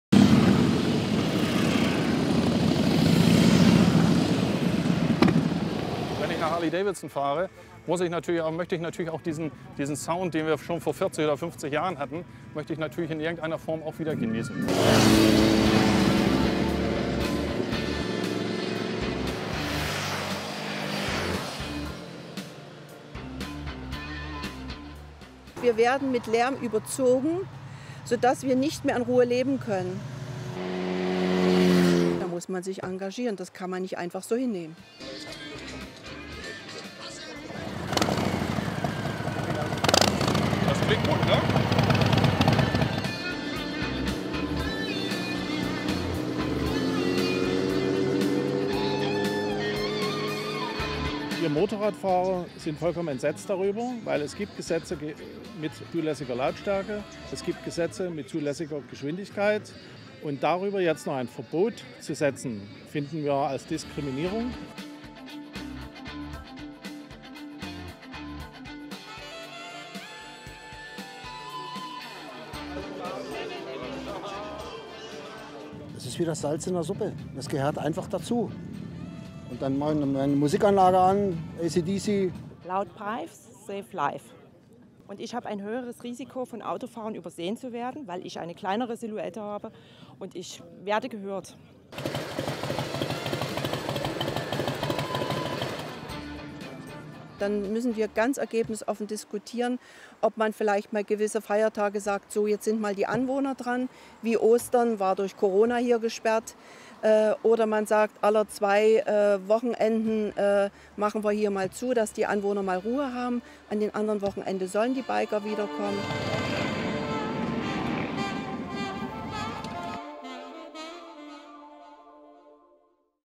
Alemania: alboroto por las motos
Los domingos suelen ser días de descanso en Alemania. Pero muchos moteros los aprovechan para poner a prueba la potencia de sus máquinas.

Un acalorado debate causa gran revuelo: ¿Deberían prohibirse las caravanas de motos durante domingos y festivos?

Cortesía de @dw_espanol